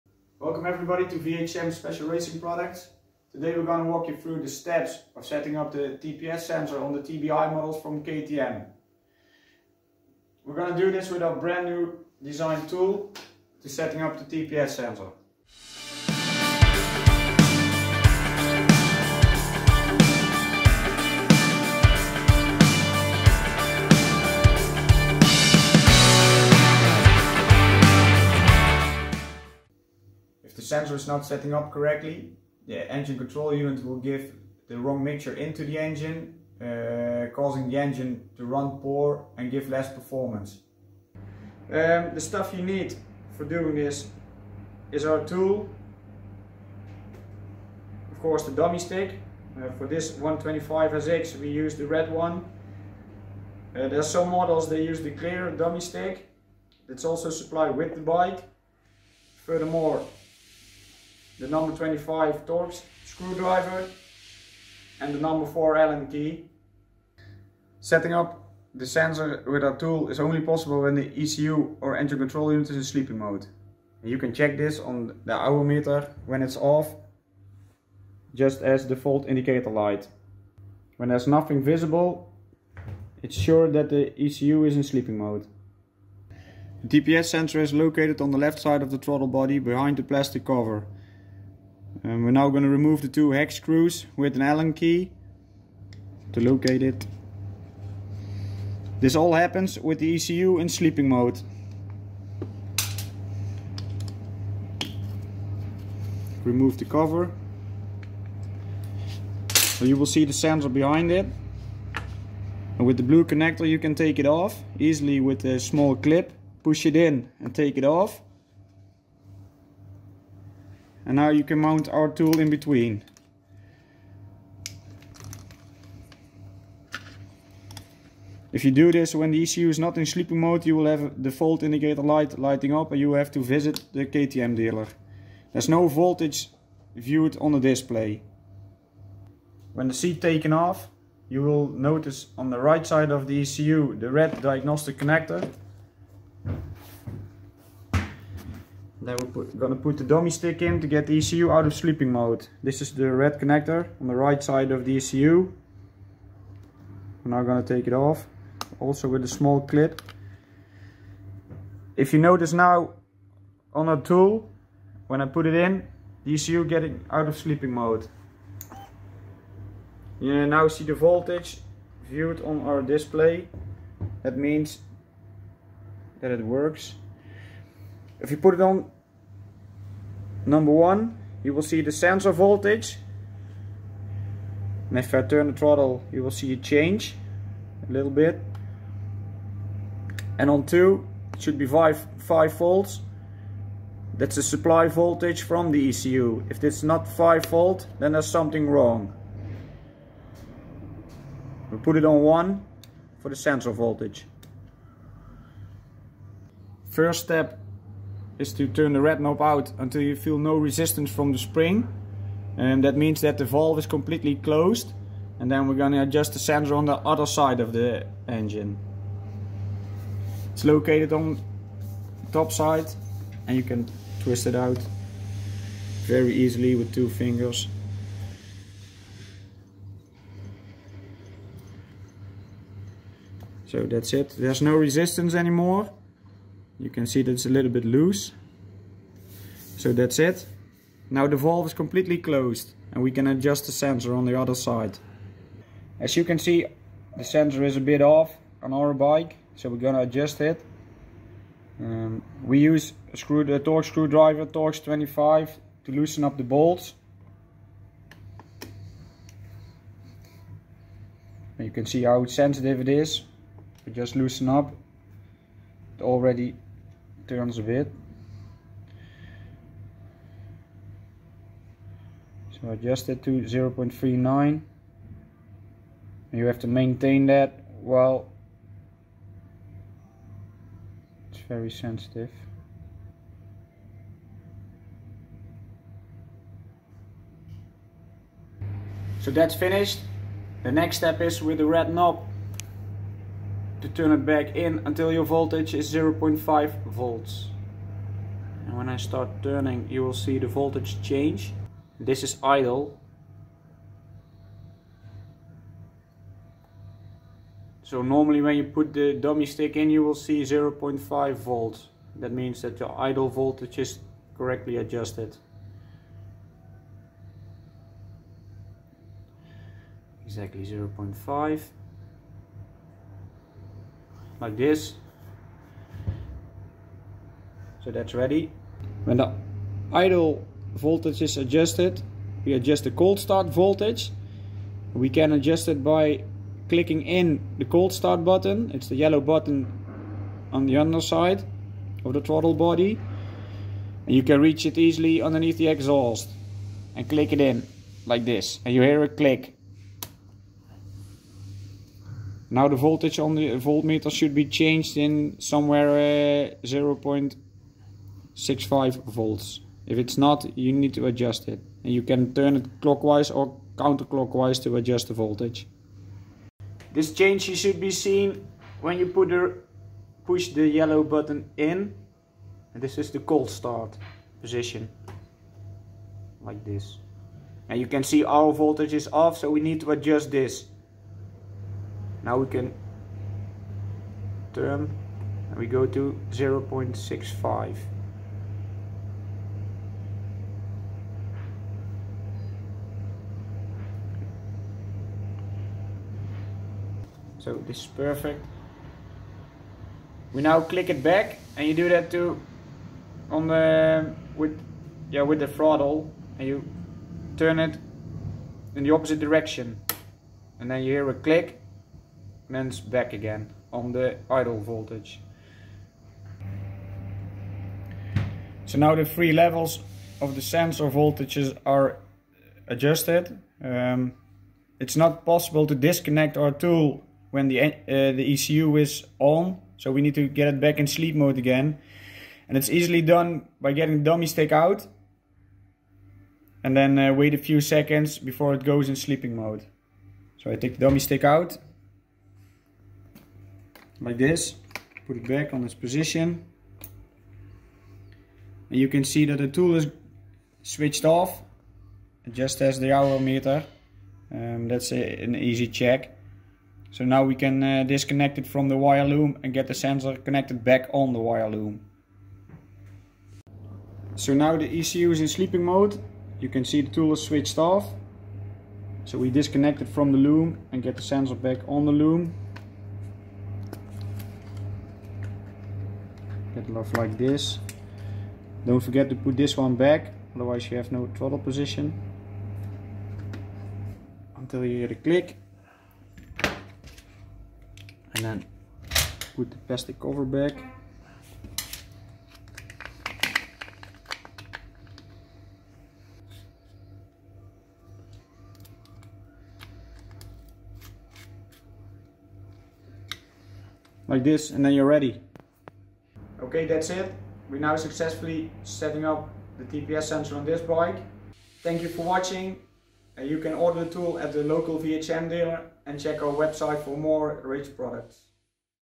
Instruction video VHM TPS setting tool
Voltages in this video (0,39V, 0,5V idle, 0,65V coldstart) are for the KTM 125SX / Husqvarna TC125 / GasGas MC125 TBI models!

Intro
Welcome everybody to VHM special Racing Products. Today we're going to walk you through the steps of setting up the TPS sensor on the TBI models from KTM. We're going to do this with our brand new designed tool to setting up the throttle position sensor (TPS).

Why check and correct the TPS?
If the sensor is not set up correctly, the engine control unit will give the wrong mixture into the engine causing the engine to run poor and give less performance.

What's needed?
VHM TPS setting tool, dummy stick (that came with the bike), 25 Torx screwdriver & #4 Allen key.

Step 1 ECU in sleeping mode
Setting up the sensor with our tool is only possible when the ECU or engine control unit is in sleeping mode. You can check this on the hour meter when it's off, just as the default indicator light. When there's nothing visible, it's sure that the ECU is in sleeping mode.

Step 2 Connect VHM tool
The TPS sensor is located on the left side of the throttle body behind the plastic cover. Remove the two hex screws with an allen key to remove the cover. You will see the sensor behind it. You can take the blue connector off easily with a small clip. Push it in and take it off. Now you can mount our VHM tool in between. There's no voltage viewed on the display yet in this step. This all happens with the ECU in sleeping mode. If you do this when the ECU is not in sleeping mode, you will have default indicator light, lighting up and you have to visit the KTM dealer!

Step 3 Use dummy plug
When the seat taken off, you will notice on the right side of the ECU the red diagnostic connector. Put the dummy stick in to get the ECU out of sleeping mode. When you put the dummy stick in, you notice that the VHM tool and ECU are getting out of their sleeping modes. This is correct. You now see the voltage viewed on the VHM tool display. If you put it on number one, you will see the sensor voltage and if I turn the throttle you will see it change a little bit. And on two it should be 5 volts. That's the supply voltage from the ECU. If it's not 5 volt then there's something wrong. Put the VHM tool on one for the sensor voltage.

Step 4 Close the valve
Now turn the red knob out of the throttle body, until you feel no resistance from the spring. It's located on top side and you can twist it out very easily with two fingers until there's no resistance anymore. That means that the valve is completely closed. Next step is to adjust the sensor on the other side of the engine.

Step 5 Adjust sensor to 0.39V
Ue use a torque screwdriver, torx 25, to loosen up the bolts of the sensor. Adjust it to 0.39V, you have to maintain that, and tighten up the bolts again. 0.39V is for the 125cc TBI models!

Step 6 Idle: sensor 0.5V
The next step is to turn the red knob back in until your voltage is 0.5 volts. When you start turning you will see the voltage change. This is idle. So normally when you put the dummy stick in you will see 0.5 volts. That means that the idle voltage is correctly adjusted. Adjusted if necessary to exactly 0.5V. 0.5V is for the 125cc TBI models!

Step 7 Clicking in coldstart
When idle voltage is adjusted you adjust the cold start voltage. Adjust it by clicking in the cold start button. It's the yellow button on the underside of the throttle body. You can reach it easily underneath the exhaust. Click it in and you hear a click. Now the voltage on the volt meter should be changed to 0.65 volts. If it's not, you need to adjust it.

Step 8 Coldstart: sensor 0.65V
If the voltage is off you need to adjust this. Turn the coldstart button and go to 0.65V. We now click it back and you do that with the throttle. Turn it in the opposite direction and then you hear a click. Then it's back again on the idle voltage. 0.65V is for the 125cc TBI models!
Use throttle to click coldstart back in position

Step 9 Remove dummy plug
So now the three levels of the sensor voltages are adjusted. It's not possible to disconnect our tool when the ECU is on. So we need to get the ECU back in sleep mode again and it's easily done by getting dummy stick out. And then wait a few seconds before it goes in sleeping mode. You can see that the tool is switched off. Just as the hour meter.

Step 10 Disconnect VHM TPS tool
Now you can disconnect the VHM tool from the wire loom. And get the sensor connected back on the wire loom. The ECU is in sleeping mode. You can see the tool is switched off. So we disconnect it from the loom and get the sensor back on the loom. Until you hear a click. And then put the plastic cover back.

We now successfully set up the TPS sensor on this bike.